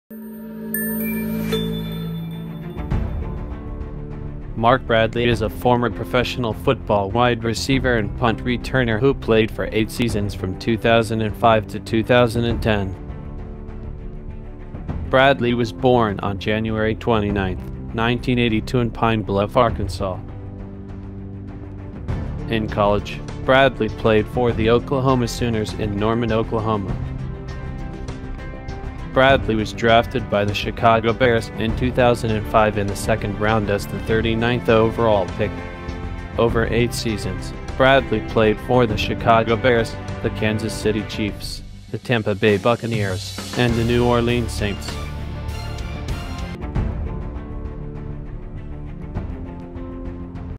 Mark Bradley: Football Wide Receiver and Punt Returner
Mark Bradley is a former professional football wide receiver and punt returner who played for 8 seasons from 2005 to 2010.

In college, Bradley played for the Oklahoma Sooners in Norman Oklahoma.

Professionally, Bradley played for the Chicago Bears, the Kansas City Chiefs, the Tampa Bay Buccaneers, and the New Orleans Saints.